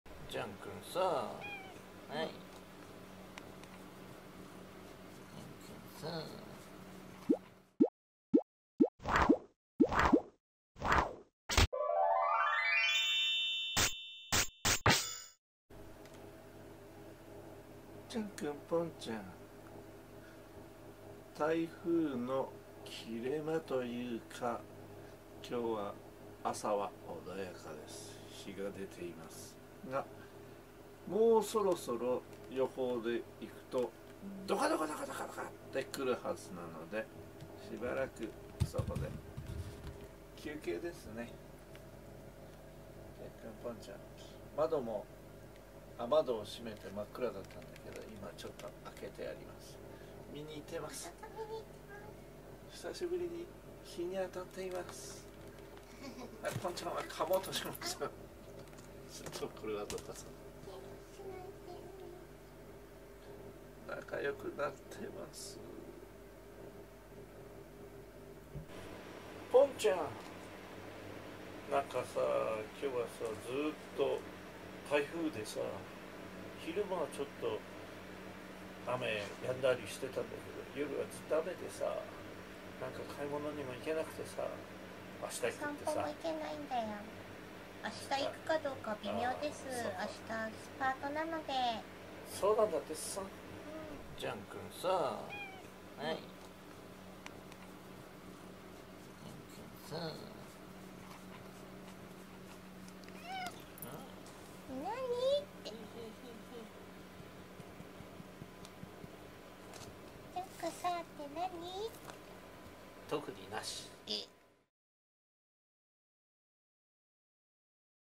ジャンくんにパパが「ただ呼んだだけ」攻撃したよ【Jean & Pont 3626】2024/08/31 保護猫ジャンけんポン
ジャンくんとポンちゃんはいつも通りです。
外は台風の切れ間で静かですがこれからどう変化するのかわかりません。
パパとママは今日の天気を何度も気にしてテレビやネットで調べまくっています。
でもジャンくんとポンちゃんはいつも通りです。